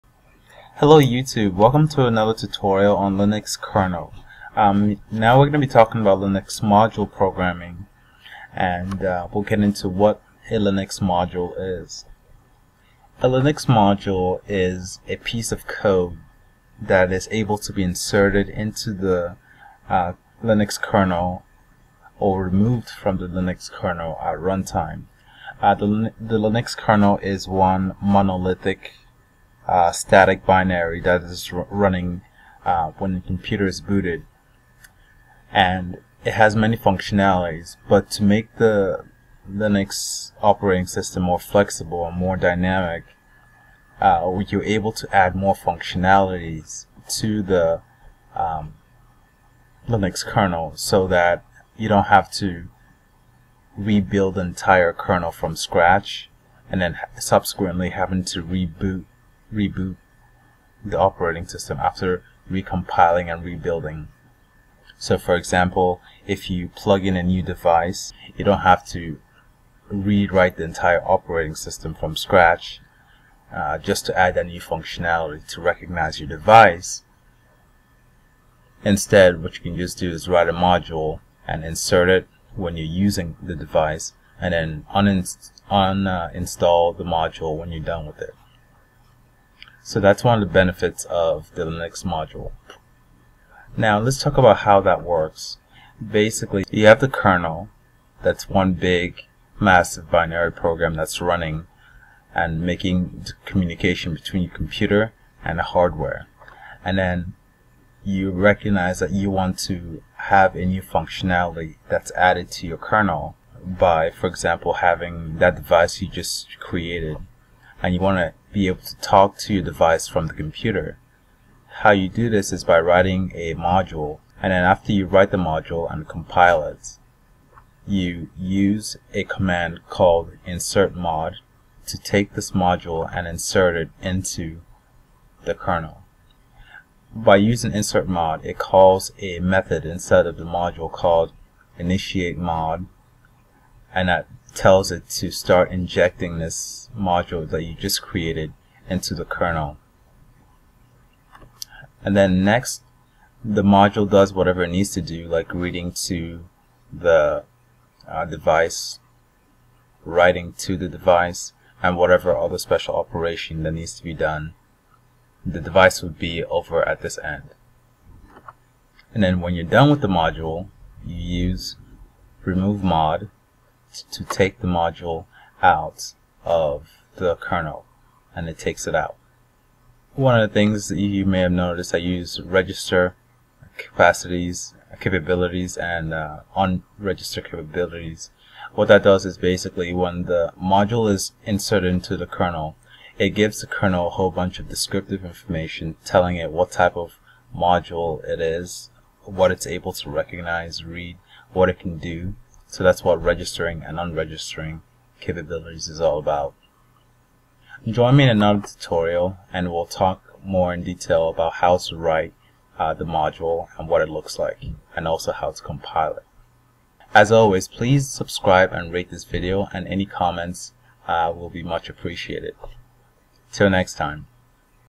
Linux Kernel Module Programming - 02
The linux kernel is "Linux".  The kernel functions to coordinate interaction between the user and the hardware by satisfying request from the user and invoking hardware communication.

This video provides introductory explaination of what the linux kernel is and how it relates to linux modules, which are pieces of code that can be added and removed to and from the kernel at runtime thereby adding increased functionality to the system.